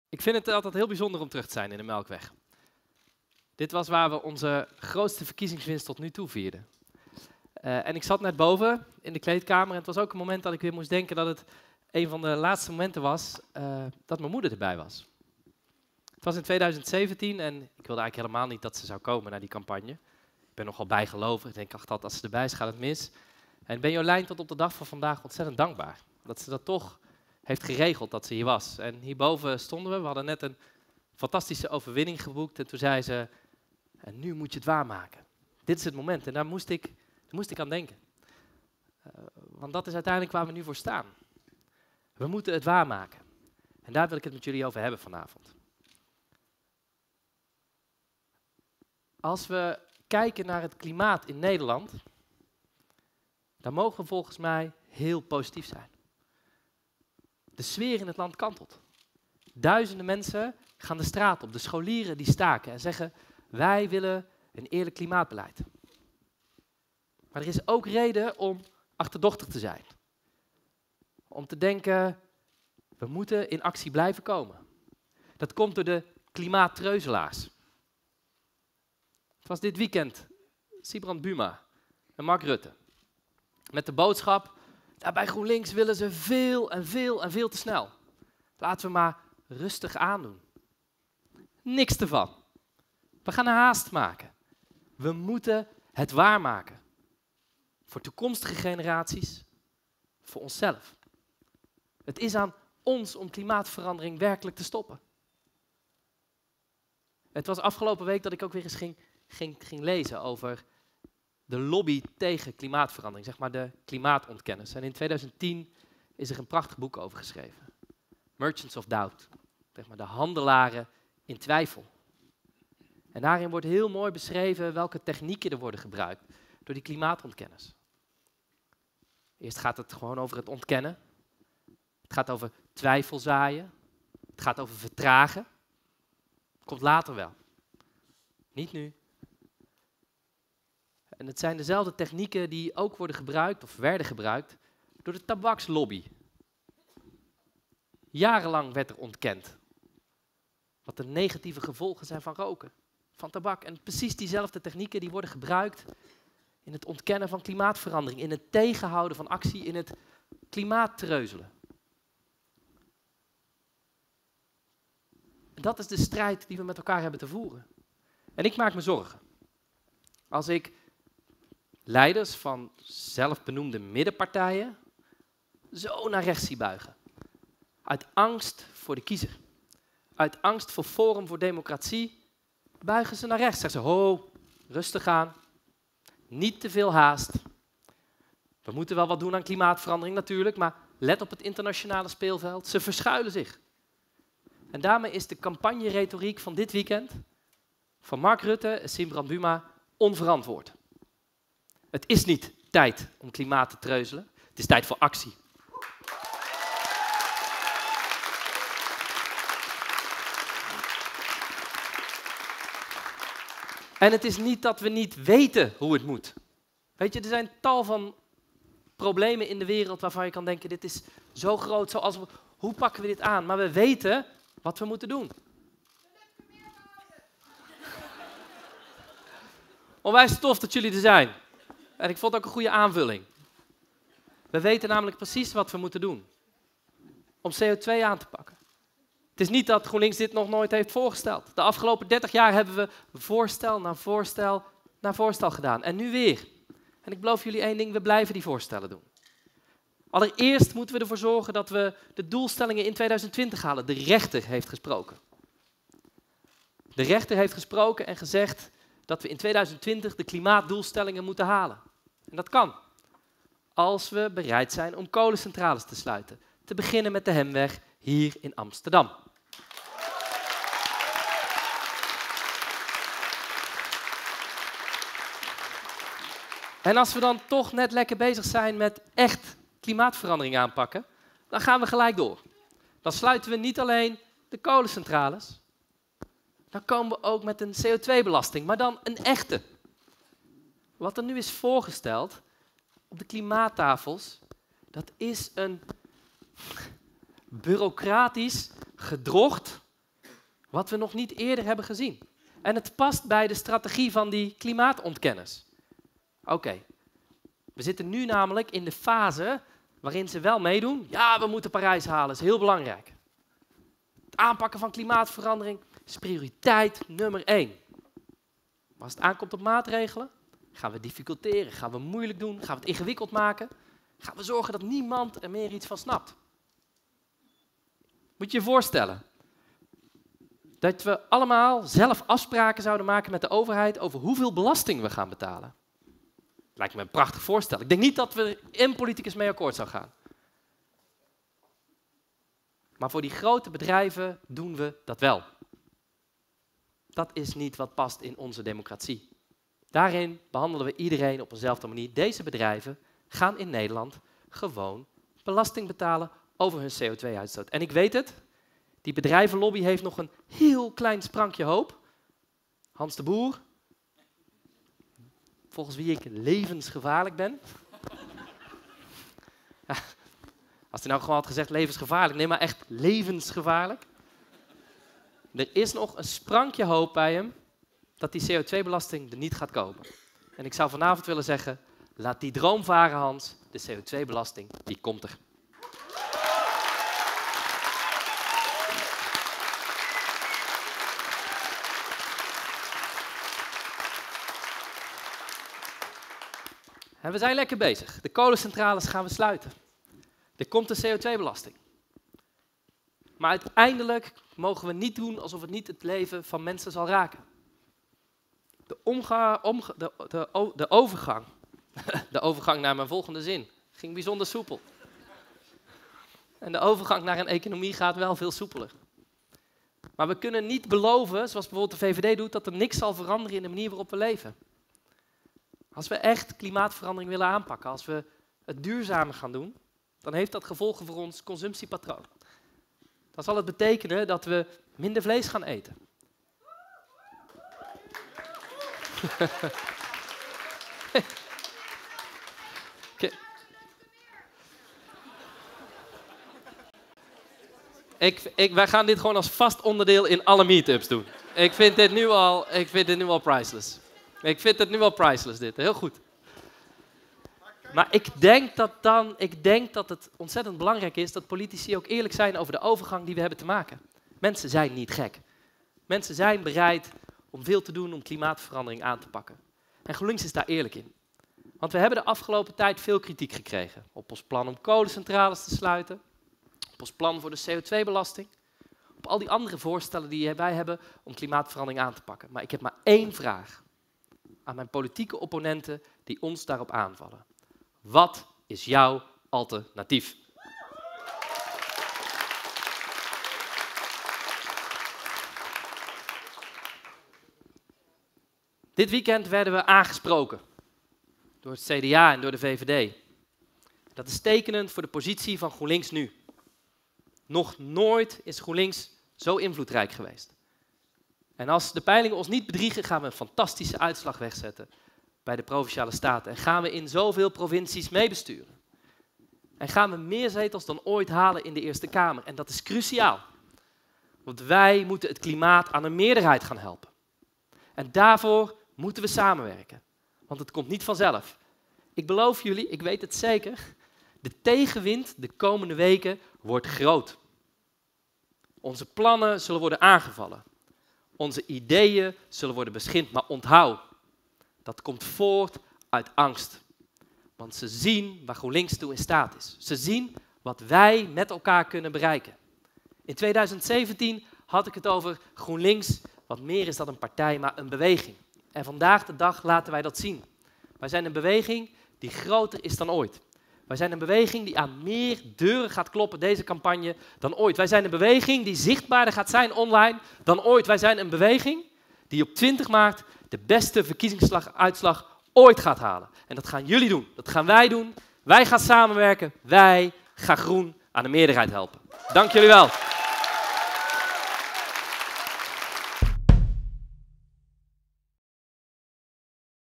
Jesse Klaver: klimaattreuzelaars, het is tijd voor actie. Meetup Amsterdam 2019
Jesse Klaver sprak op de Meetup in de Melkweg Amsterdan. In zijn speech onderstreepte hij de haast die er is om maatregelen te nemen tegen klimaatverandering. De stille meerderheid in Nederland wil vooruit maar partijen als VVD en CDA staan op de rem. We moeten niet treuzelen. We moeten in actie. Om de klimaatdoelen te halen en de hogere energierekening van mensen te compenseren. Om te beginnen met een CO2-belasting voor grote bedrijven.

Stem 20 maart voor verandering. Stem 20 maart voor het klimaat.